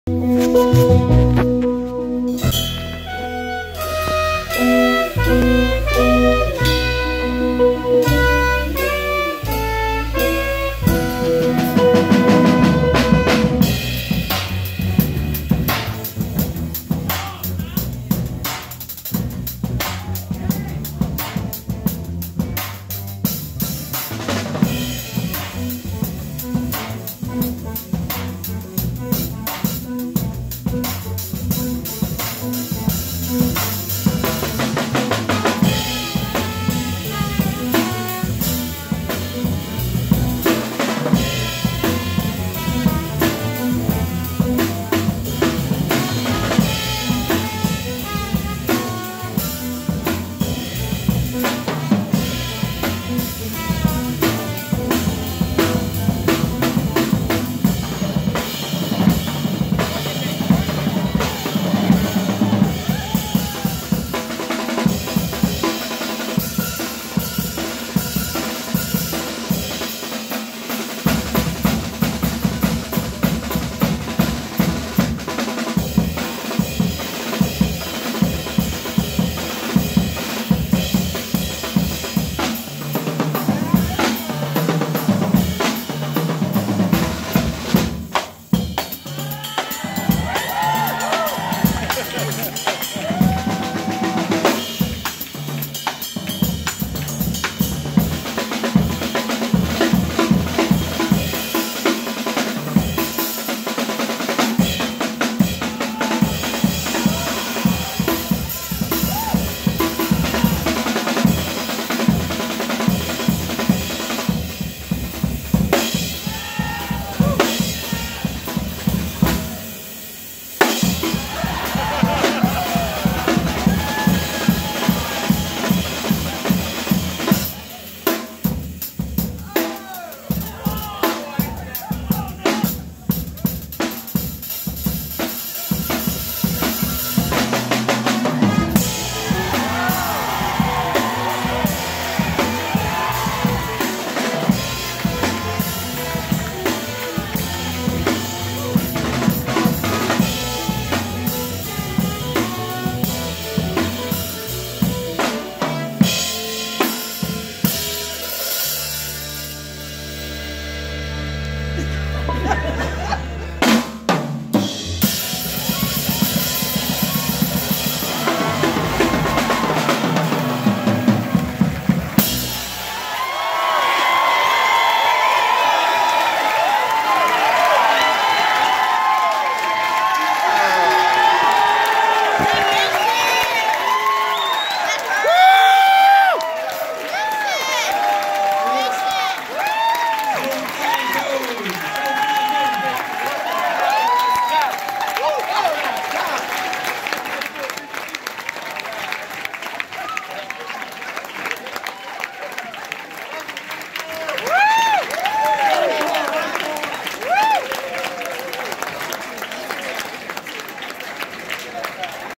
Mason Cox drum solo 2018.05.21
Community High School  2018 band So Fly Zone
a The Ark in Ann Arbor, MI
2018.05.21
Mason Cox - Drums
Jake Lee - Trumpet
Aris Chalin - Piono
Danny Freiband - Guitar
Timmy Walters - Bass
Jack Wagner